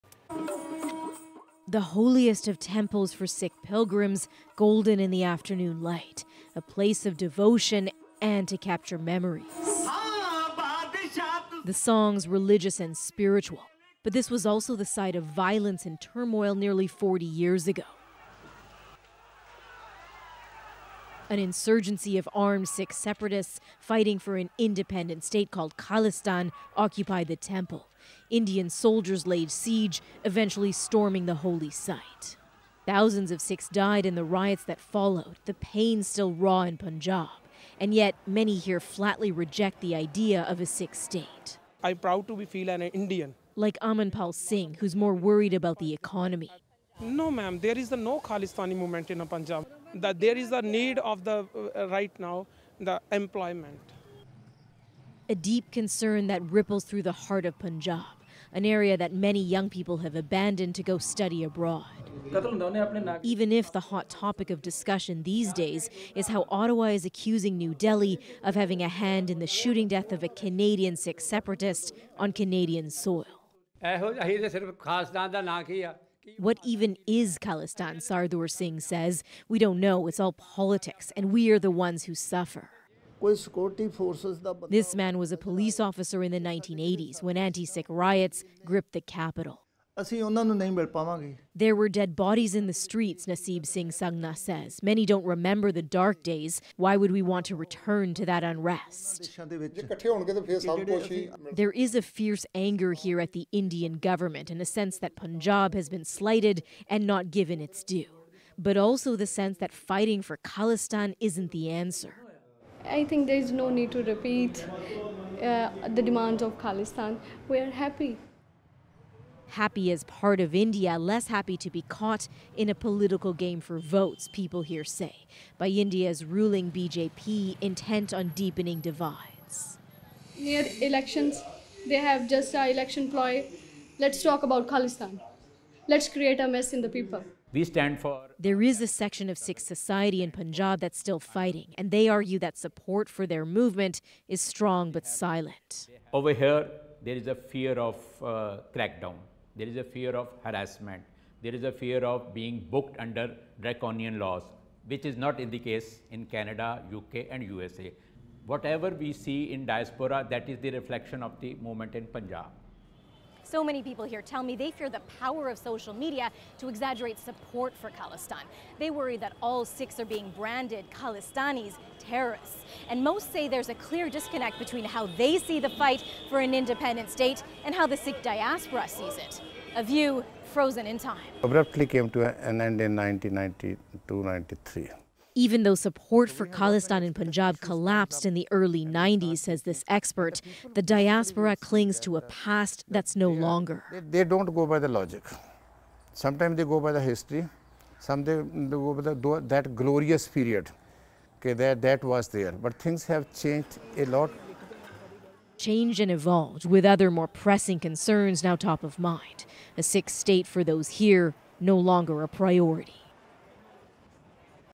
Why Sikhs in India fear Khalistan support is being exaggerated
On the streets of Punjab, many Sikhs reject the idea of a separate Khalistan and fear the idea is being exaggerated on social media. CBC’s South Asia correspondent Salimah Shivji breaks down why.